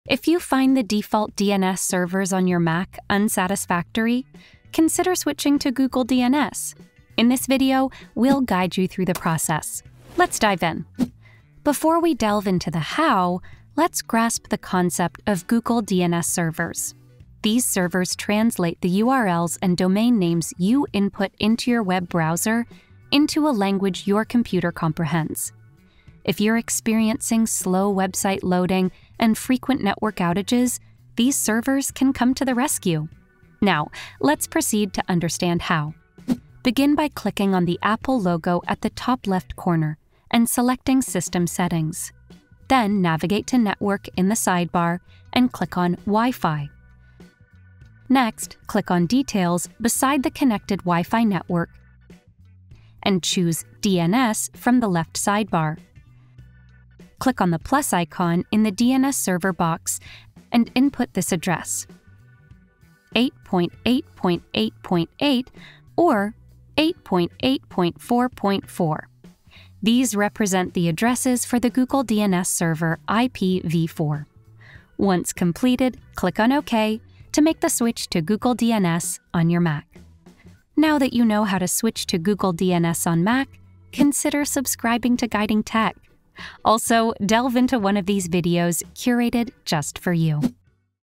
How to Switch to Google DNS on Mac | What is Google DNS? | 2024 Tutorial
What is Google DNS, and how can it benefit your Mac? Interested in switching to Google DNS on your Mac but unsure how to do it?

In this video, we cover everything you need to know about Google DNS.

Don't forget to explore one of these videos for even more tech enjoyment!

Input this address: 8.8.8.8 or 8.8.4.4

00:00 - Intro
00:11 - What are Google DNS servers
00:36 - How to Switch
01:20 - Outro